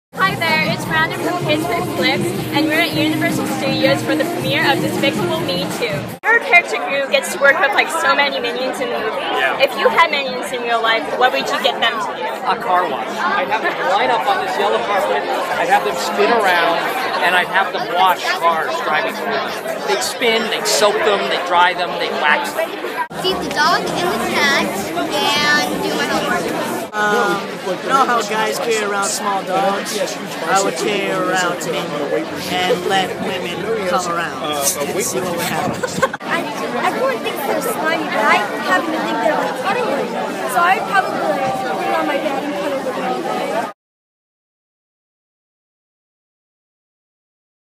DESPICABLE ME 2: What Would You Do with Minions?
Steve Carell & the stars of DESPICABLE ME 2 tell KidsPickFlicks.com's Miranda what they would do if they had their own Minions.